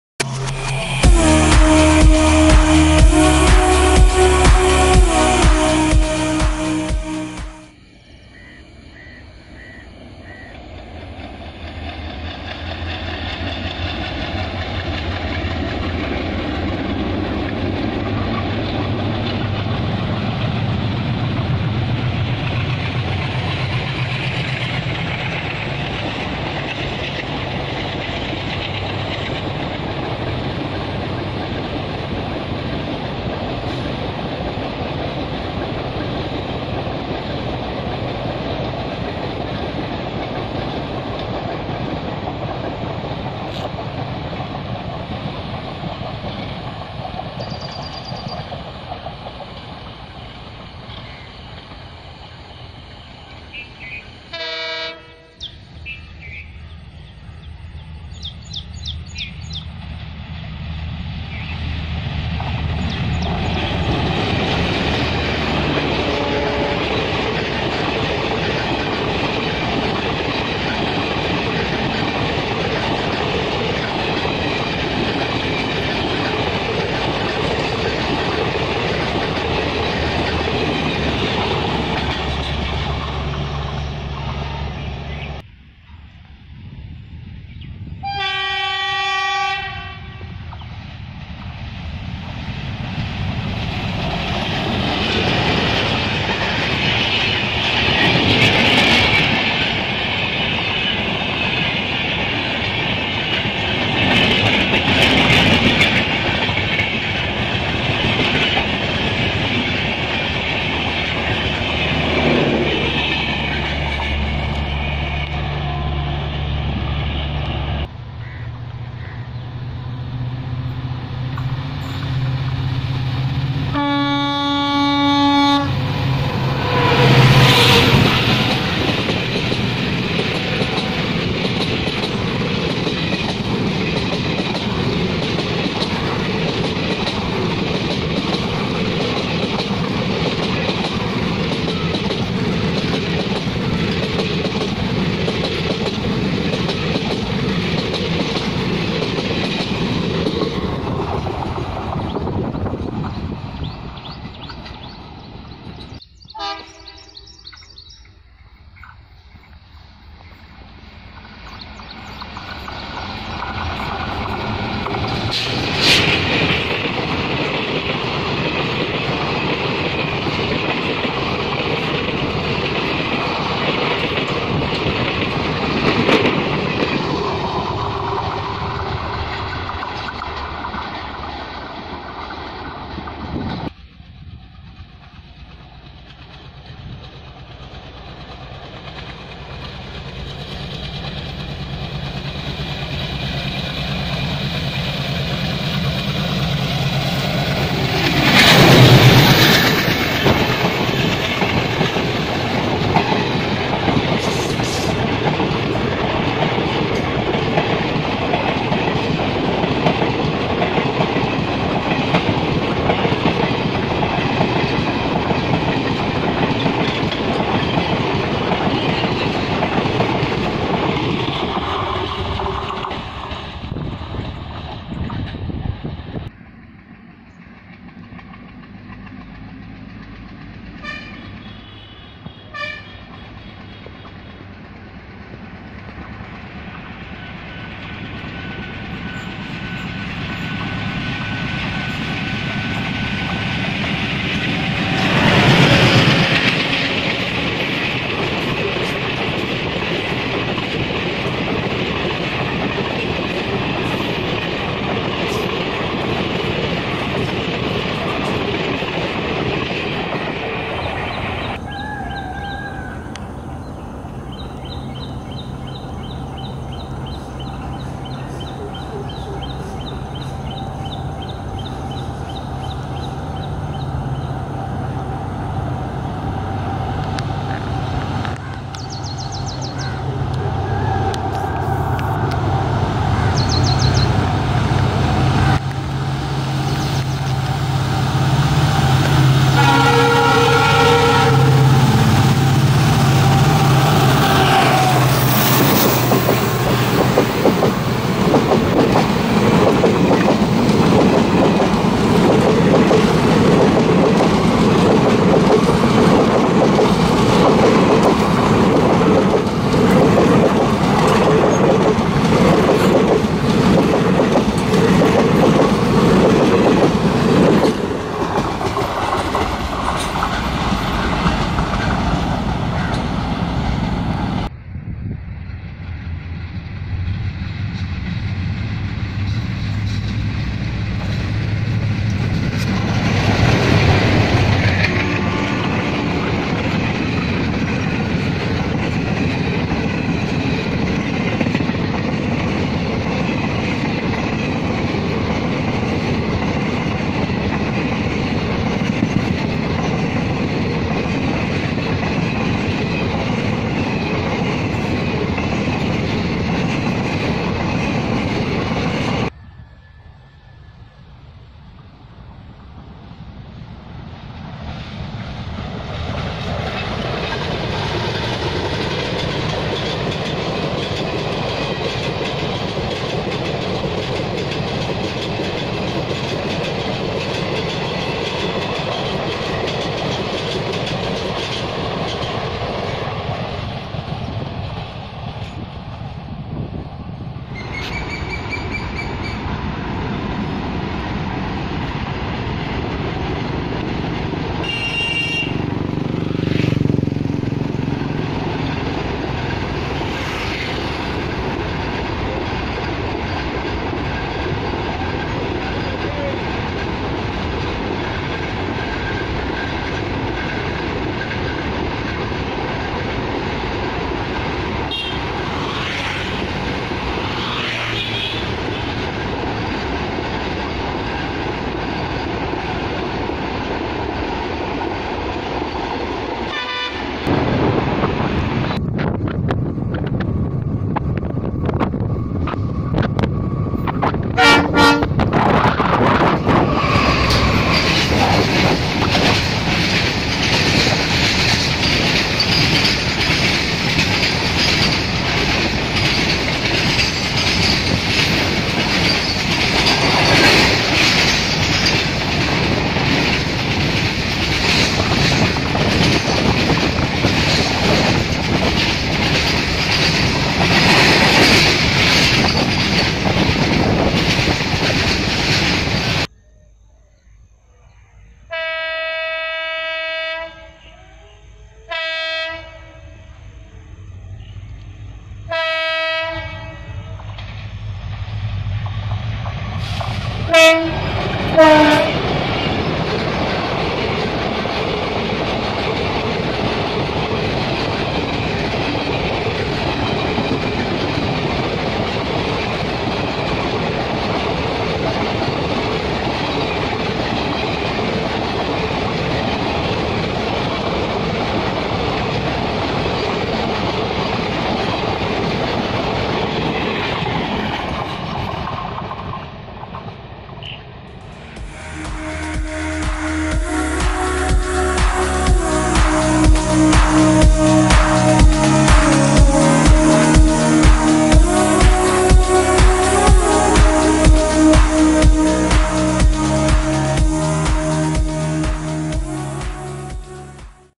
[13 IN 1] MONSOON KONKAN RAILWAY: CBE BKN+DURANTO+ROCKET+WEEKLY+PASS+BCNA
[13 IN 1] MONSOON KONKAN RAILWAY:COIMBATORE BIKANER+DURANTO+ROCKET+WEEKLY+PASS+BCNA

1.22113 LTT-KOCHUVELI SF EXP
   KALYAN (KYN) TWINS WDM3D#11455 + #11358

2.22476 COIMBATORE-BIKANER AC SF EXP
    ABU ROAD (ABR) WDM3A#16838R

3.12224 ERNAKULAM-LTT DURANTO EXP
    KALYAN(KYN)WDM3D#11362

4.12133 MUMBAI CSTM-MANGLORE SF EXP
    ERODE(ED)WDG4#12652 +KALYAN(KYN) WDM3D#11398 DEAD

5.22636 MAQ-MAO ICE EXP
   ERNAKULAM(ERS) WDM3A#16565

6.19260 BHAVNAGAR-KOCHUVELI EXP
    RATLAM (RTM) WDM3D#11482

7.56641 MAO-MAQ PASSENGER
    ERODE(ED) WDM3D#11201

8.12133 MUMBAI CSTM-MANGLORE SF EXP
    KALYAN (KYN) WDP4D#40347

9.16337 OKHA-ERNAKULAM EXP
    ERNAKULAM (ERS) TWINS WDM3A#14125 + #16405

10.56640 MAQ-MAO PASSENGER
     ERODE (ED) WDM3D#11223

11.GARUD 45/870 BCNA RAKES
     KRISHNARAJAPURAM(KJM) WDG3A#13053

12.MANGAL 49/640 BCNA RAKES
      ERODE(ED) WDG4#12807

13.MANGLORE-LTT MATSYGANDHA SF EXP
      ERODE (ED) WDG4#12807

VIDEO BY,
PRAVIN SOLANKI